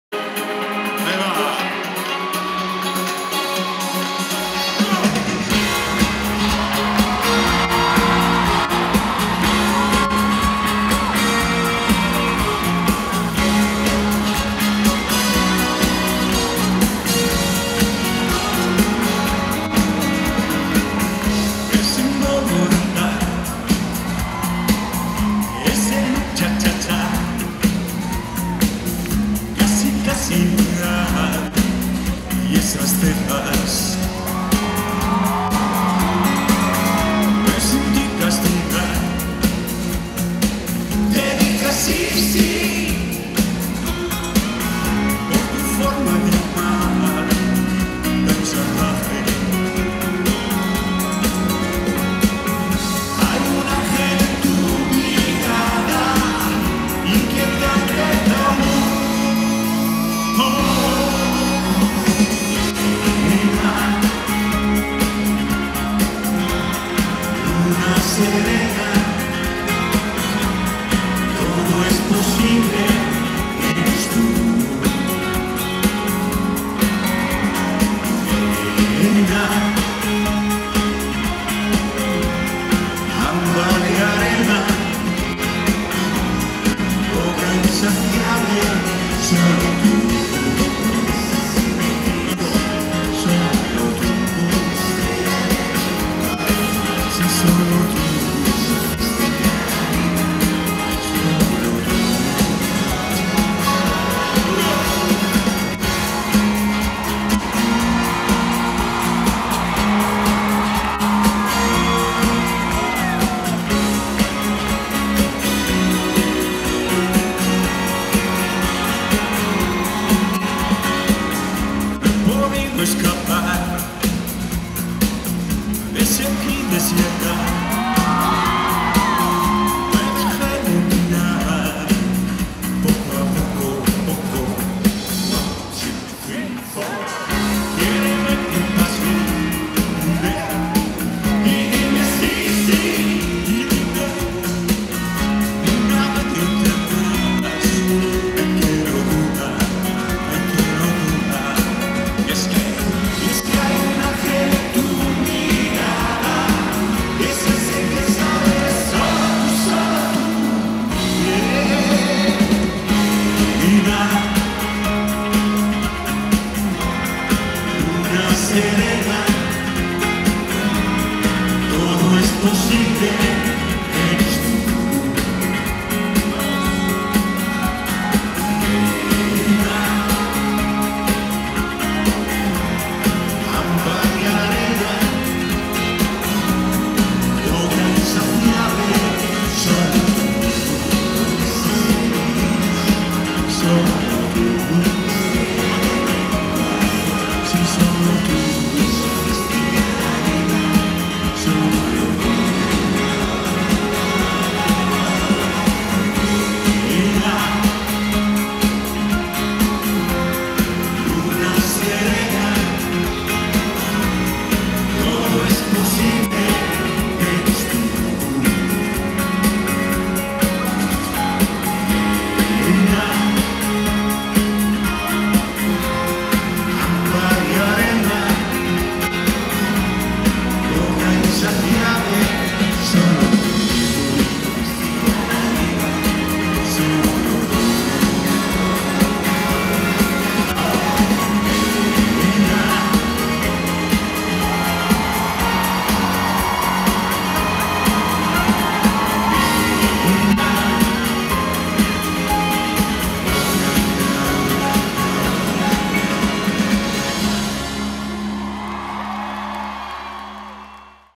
MIGUEL BOSE - CONCIERTO EN PUERTO RICO 2017
Miguel Bosé regresa a suelo boricua cautivando una vez mas a su fanaticada.

Video: Jaime Zeno
Produccion: Cesar Seinz
El Video aquí presentado fue previamente autorizado por la producción del artista